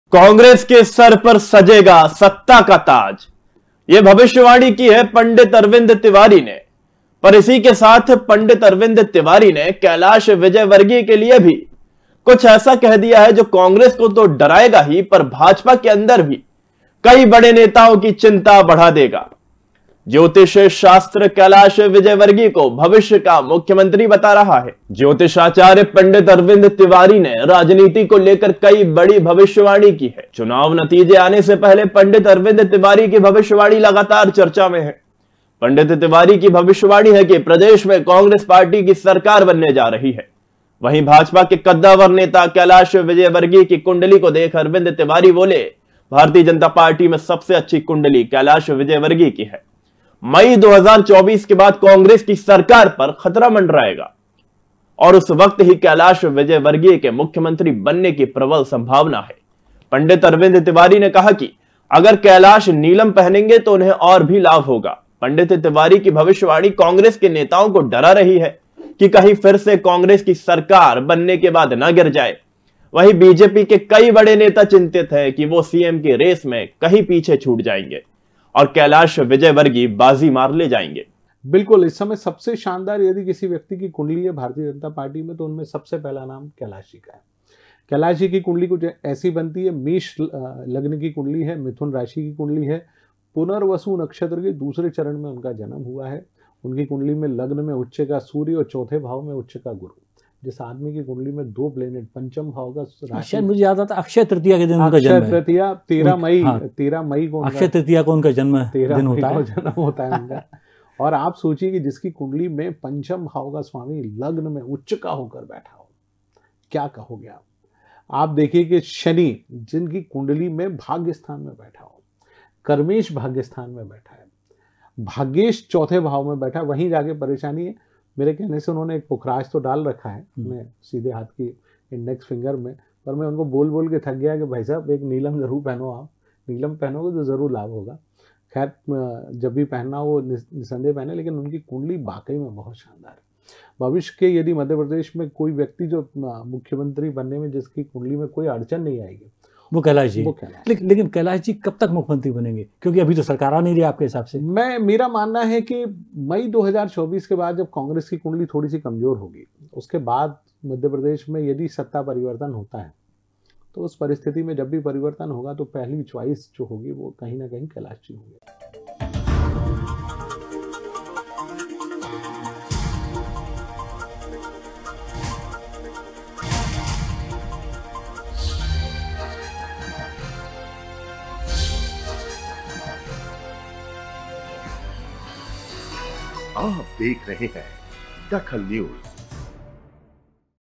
DAKHAL NEWS पंडित अरविंद तिवारी की भविष्यवाणी
पंडित अरविंद तिवारी की भविष्यवाणी
कैलाश विजयवर्गीय  बनेंगे मुख्यमंत्री